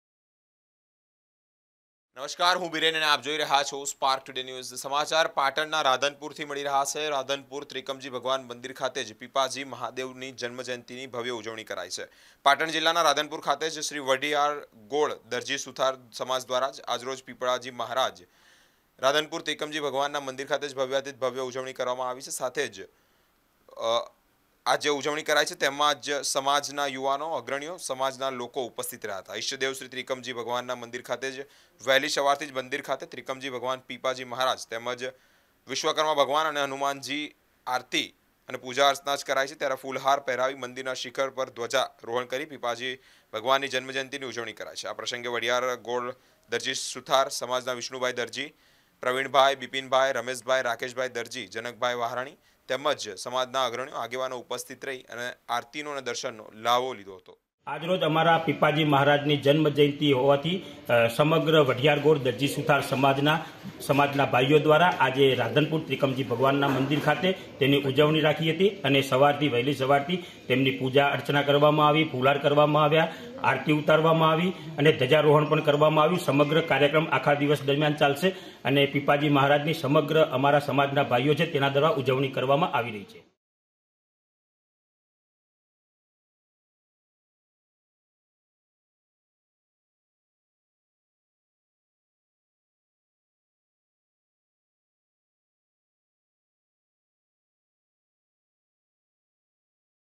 રાધનપુર ત્રિકમજી ભગવાન મંદિર ખાતે પીપાજી ભગવાનની જન્મ જયંતીની ભવ્ય ઉજવણી કરાઇ 2023
રાધનપુર ત્રિકમજી ભગવાન મંદિર  ખાતે પીપાજી ભગવાનની જન્મ જયંતીની ભવ્ય ઉજવણી કરાઇ